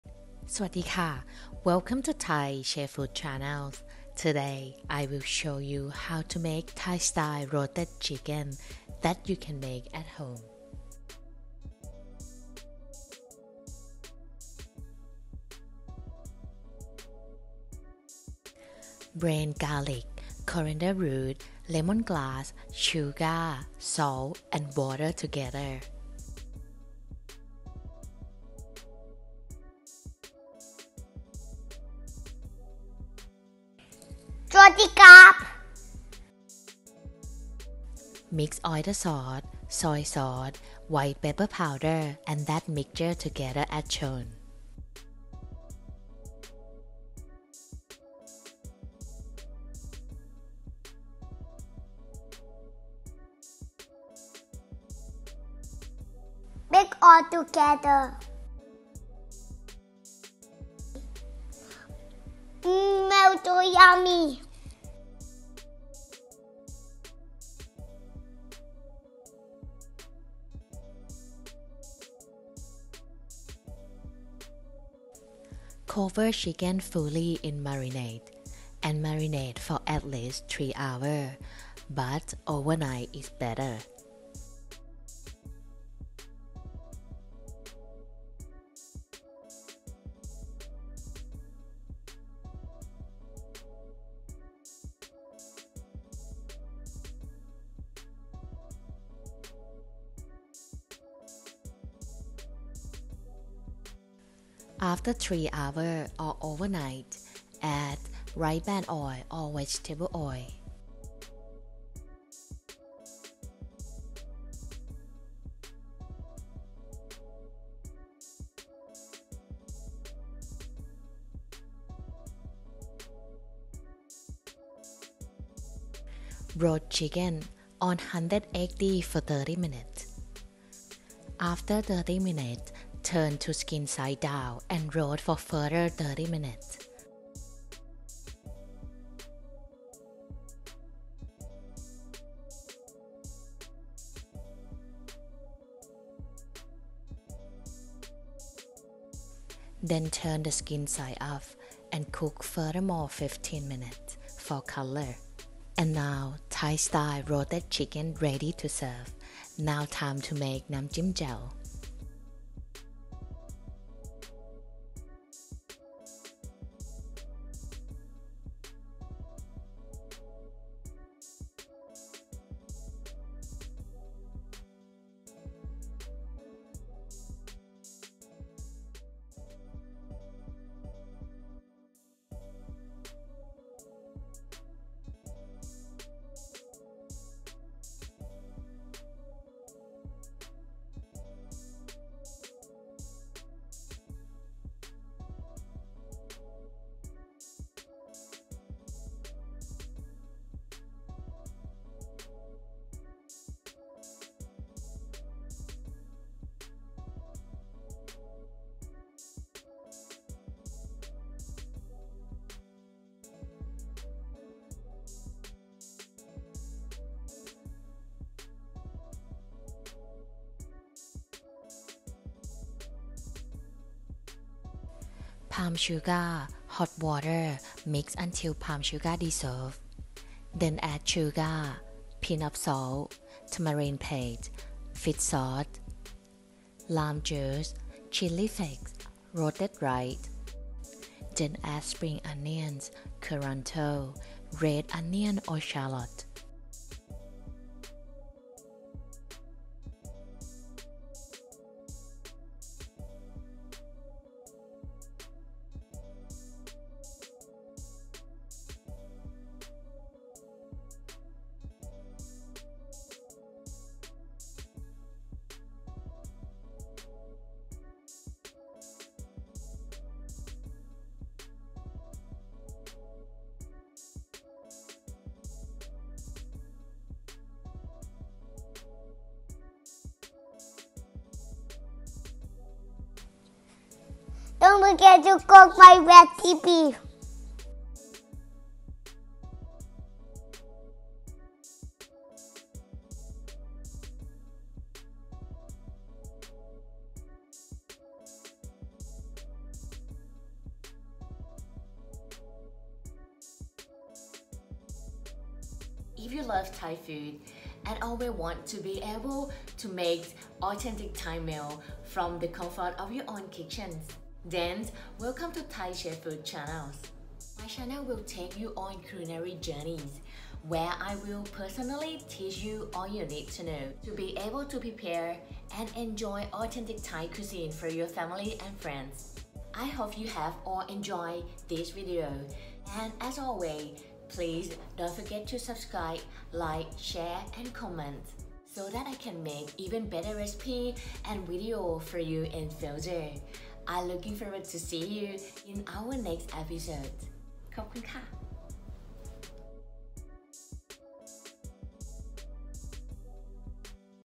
Amazing Roasted Thai Style Chicken• ONE OF THE RECIPES that you can eat in Thailand|ThaiChef Food
🟠ingredients
Whole Chicken
30g. Garlic
1 pc. Lemongrass
5 pcs. Coriander root
20 g. Sugar
1/4 tsp. Salt
1 cup. Water
80 g. Oyster sauce
160 g. Soy sauce
1 tbsp. White pepper powder
50 ml. Rice brand oil

🟠Sauce Recipe
25 g. Palm sugar
2 tbsp. Water
1/2 tsp. Sugar
Pinch of salt
1 tsp. Tamarind paste
2 tbsp. Fish sauce
1.5 tbsp. Lime juice
Chili flakes
Roasted rice
Spring onion
Culantro
Red onion or shallot